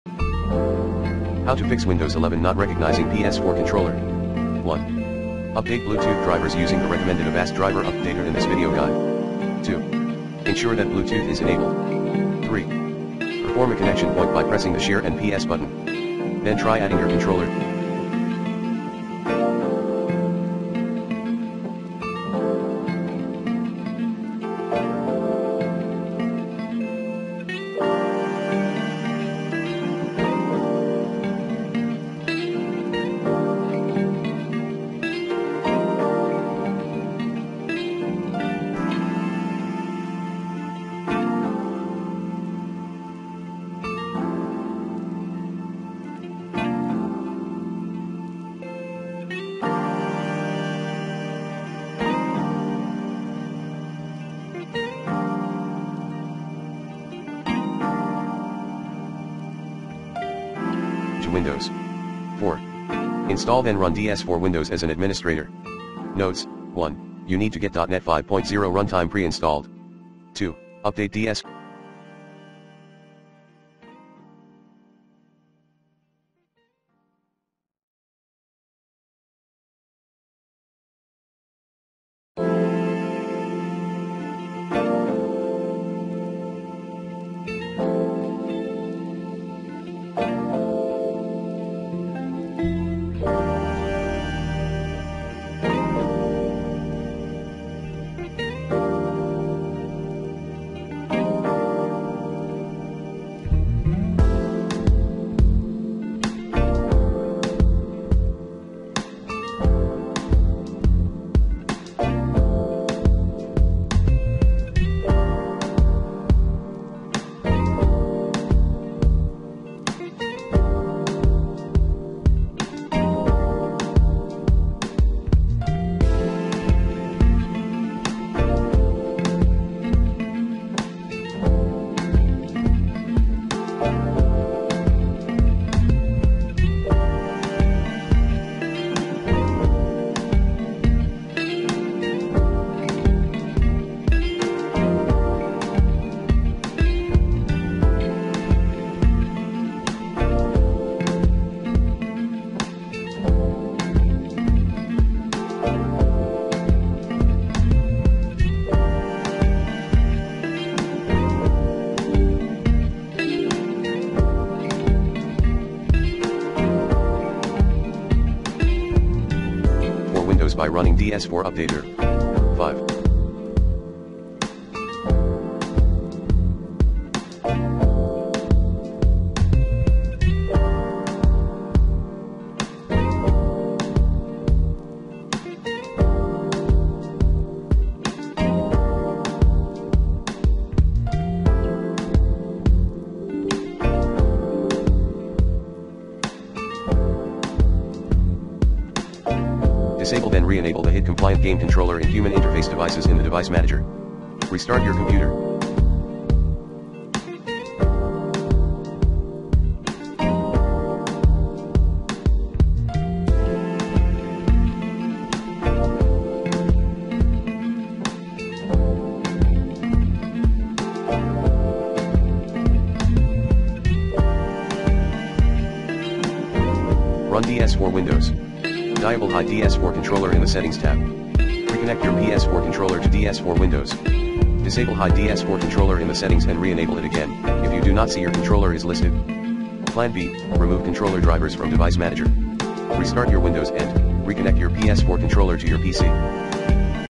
Fix Windows 11 not recognizing PS4 controller
Here's how to Fix Windows 11 not recognizing PS4 controller. Run Avast Driver Updater@ to keep all your device drivers up-to-date effectively.